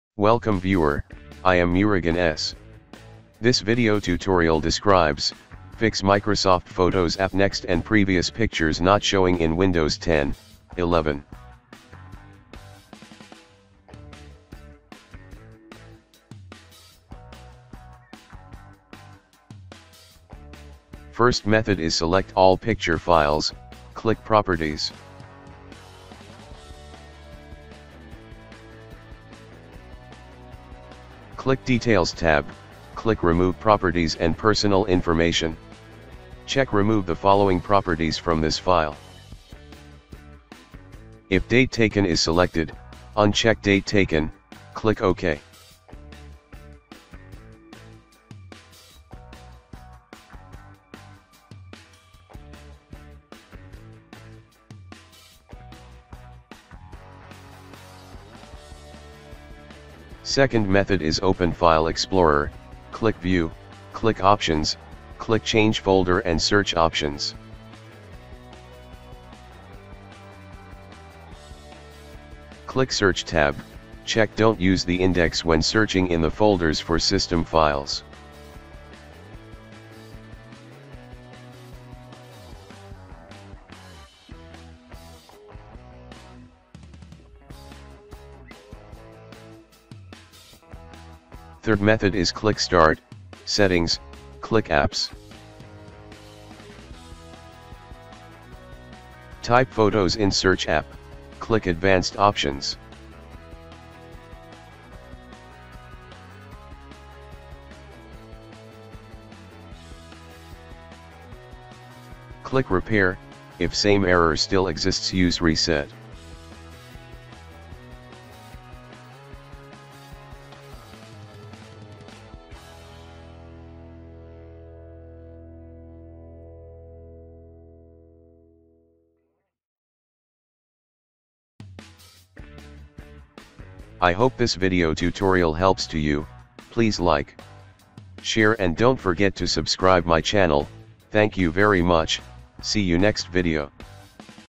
Fix Microsoft Photos App Next and Previous Pictures Not Showing In Windows 10/11
This Tutorial Helps to Fix Microsoft Photos App Next and Previous Pictures Not Showing In Windows 10/11

00:00 Intro
00:12 Method 1 - Change Image Properties
00:59 Method 2 - Change View Options
01:31 Method 3 - Reset & Repair
02:14 Closing